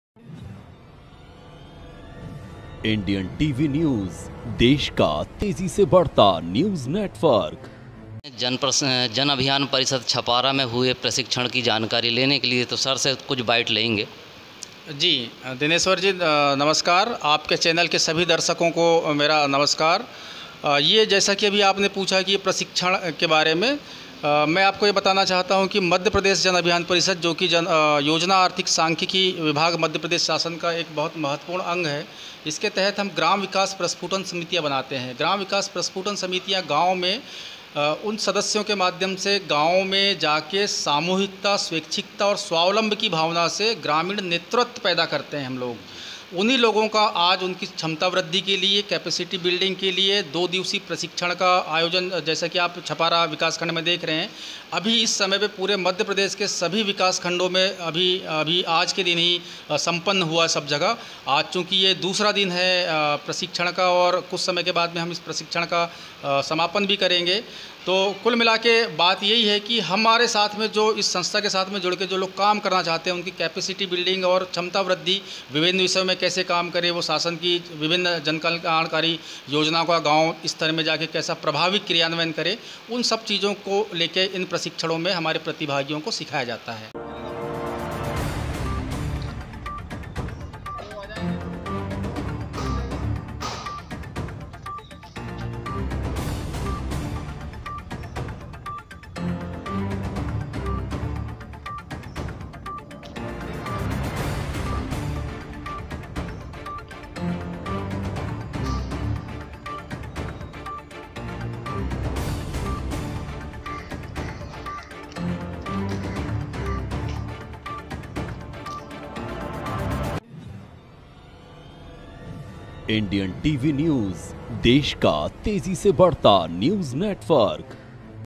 सिवनी एमपी के विकास खंड छपारा द्वारा दो दिवसीय प्रशिक्षण कार्यशाला का आयोजन हुआ
जिला सिवनी के छपारा जनपद में  मध्यप्रदेश जन अभियान परिषद (योजना आर्थिक और सांख्यिकी विभाग मध्यप्रदेश शासन) विकास खंड छपारा द्वारा ग्राम विकास प्रस्फुटन समिति की क्षमता व्रद्धि हेतु दो दिवसीय प्रशिक्षण कार्यशाला का आयोजन स्थानीय जनपद सभाकक्ष में प्रारंभ हुआ